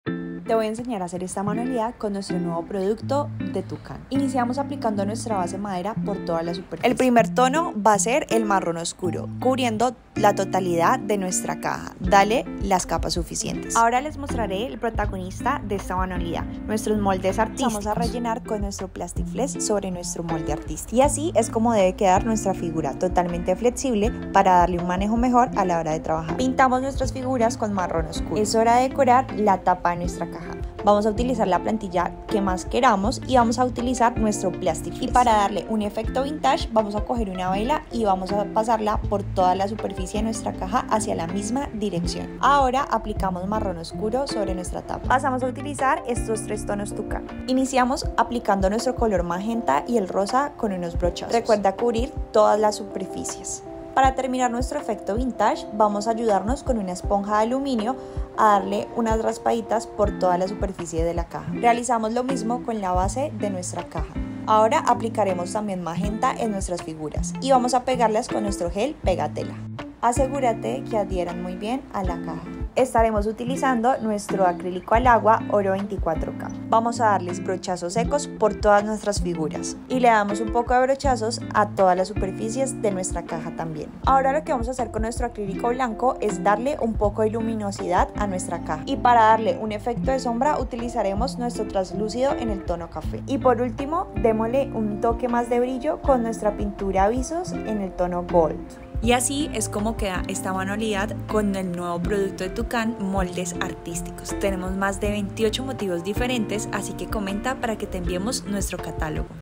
Como decorar una caja con moldes artisticos - Pinturas Tucan
Aquí te dejamos una idea para que pintes una cajita y la decores con los ornamentos que puedes sacar con nuestros moldes artísticos.